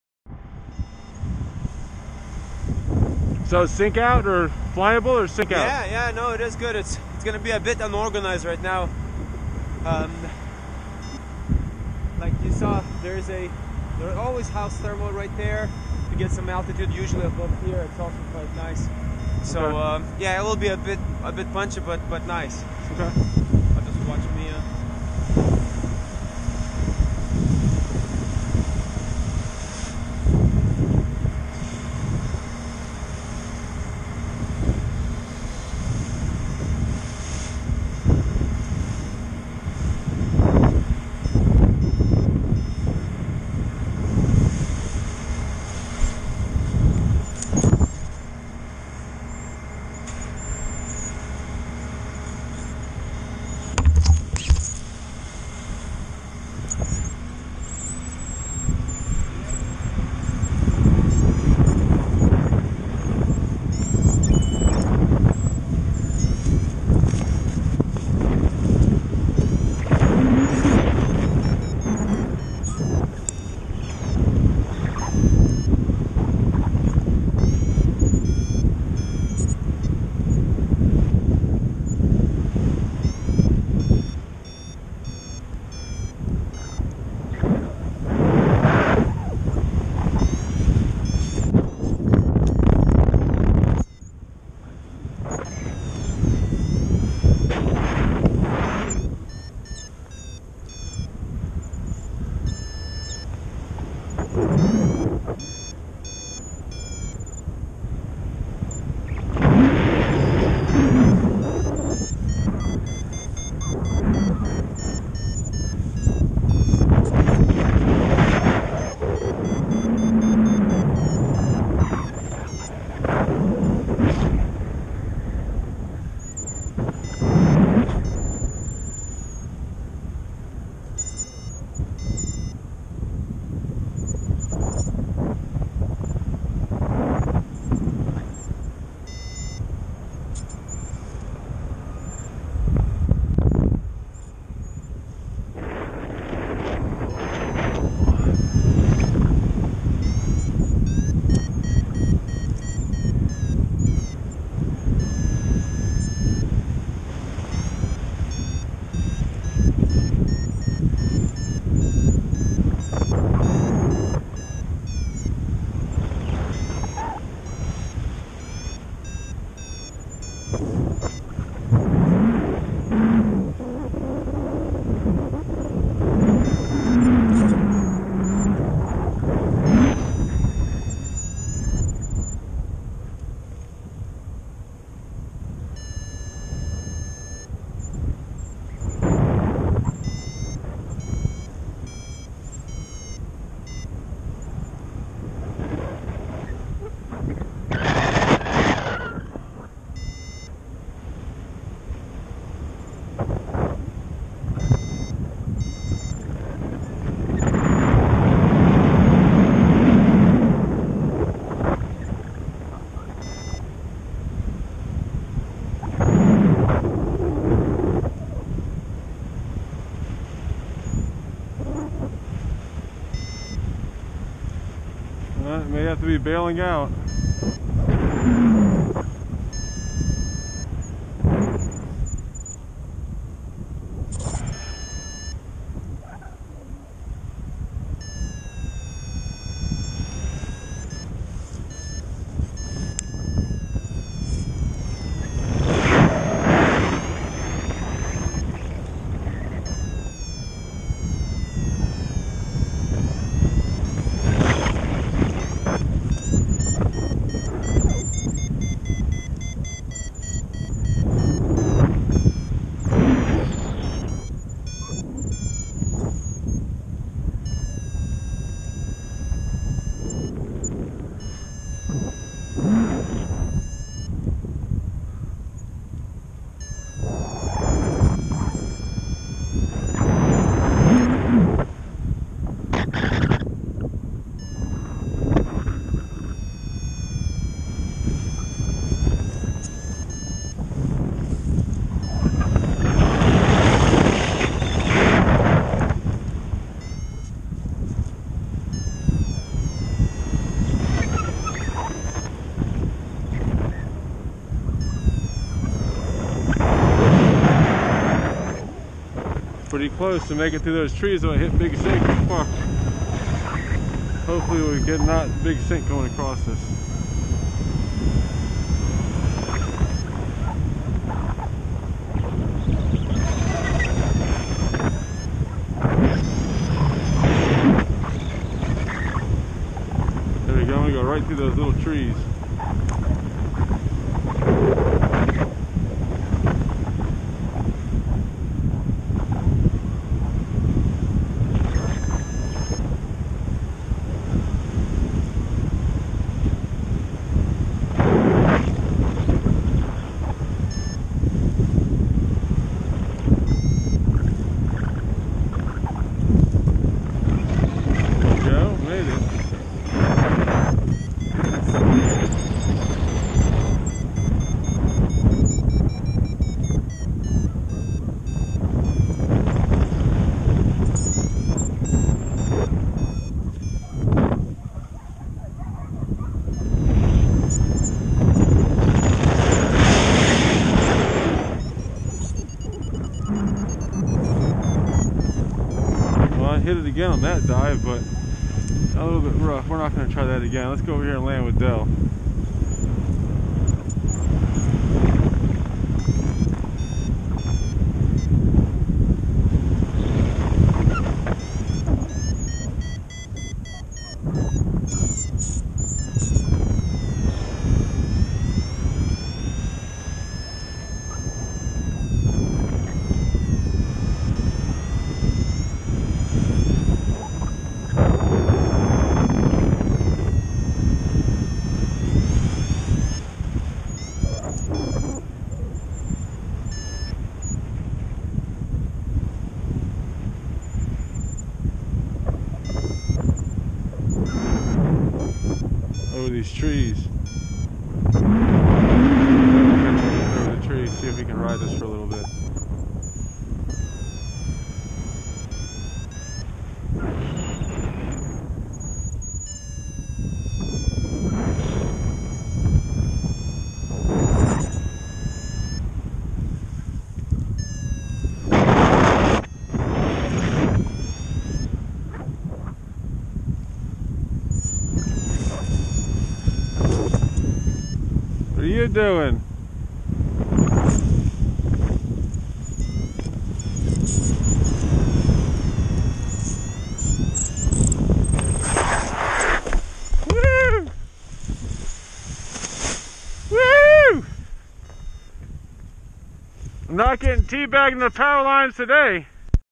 Power Line Coral
My first Sky Retreat Launch with my Tequila 4.

The air was pretty active with punchy hit or miss thermals; tons of sink between. After I get low I make a run for the LZ, barely make it over a tree line then see Del landing in the distance. I say out loud that I'm going to go land over with Del not knowing that he had a sketch landing and was right at the base of power lines.

As I approach and fly over the pine trees at LZ I'm sure there is a connecting power line across the field but I cant see anything and Del is right there; I keep looking but see nothing.

I get out of my pod (drop the landing gear) and say to Del, "what are you doing?". When I look up the power lines are about 15' directly in front of me! I didn't have time to process what was going on; only enough time to react; I crank a hard 90 degree left turn only to be confronted with another set of power lines (which I had clearly seen) so I had to make another 90 degree turn over a barb wire fence and landed down wind on the road (no traffic).

No power line T-Bag for me!

Always come into LZ with plenty of speed. just in case it's needed for a maneuver like this.

Lots of mistakes on this flight I wont be making again.